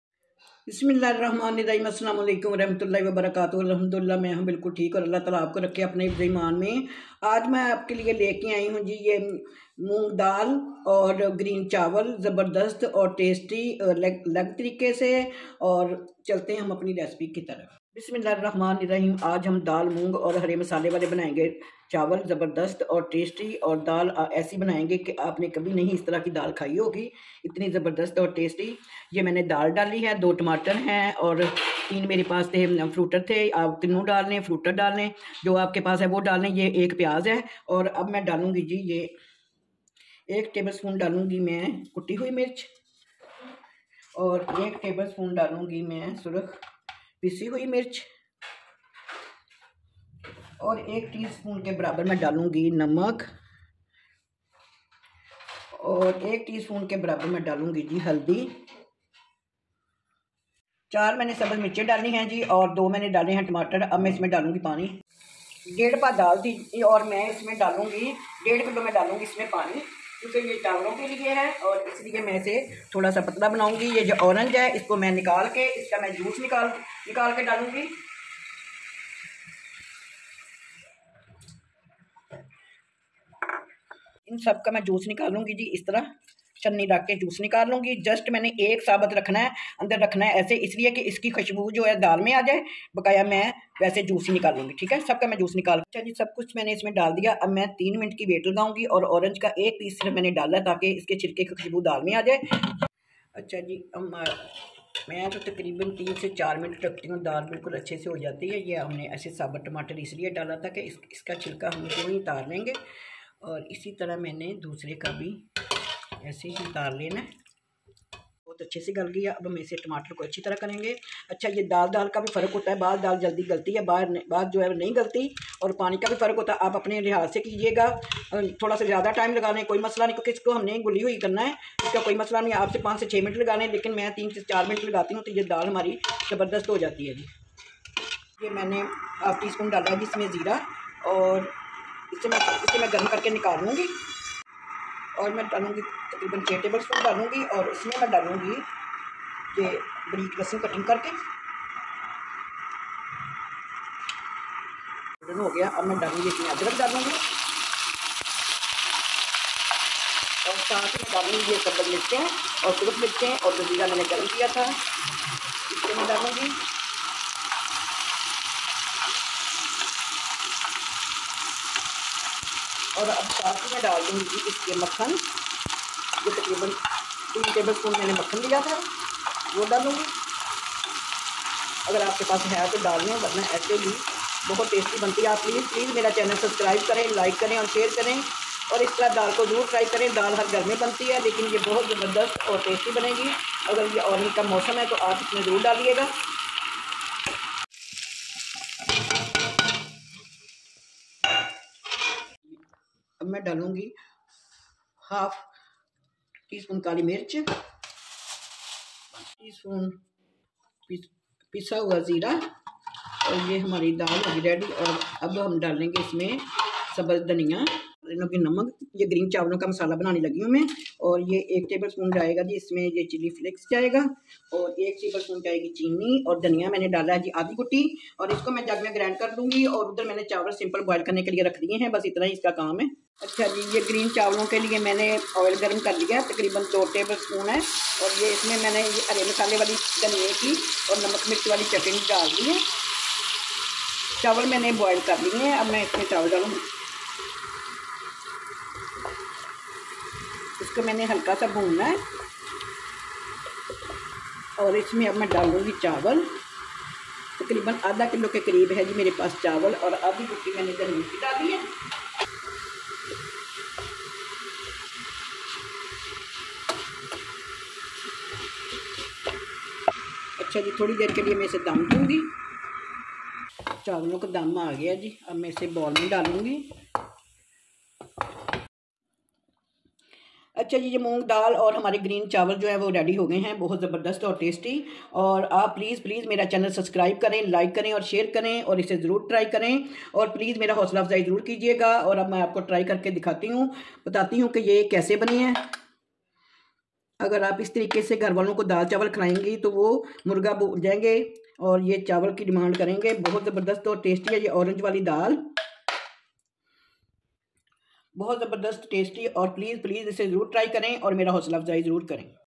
Orange Moong Daal With Green Masala Rice Recipe | Mazedar Daal Chawla Recipe
Ingredients
Daal 1&half pao
Oranges 3
Tomato 2
Onion 1
Green chilli 4
Red Chilli flakes 1 tbs
Coriander
Tadka ingredients:
Cumin half tsp
Red Chilli
Green chilli

Green Rice Ingredients
Coriander 1 bowl
Salt 1 tsp
Boiled rice half kg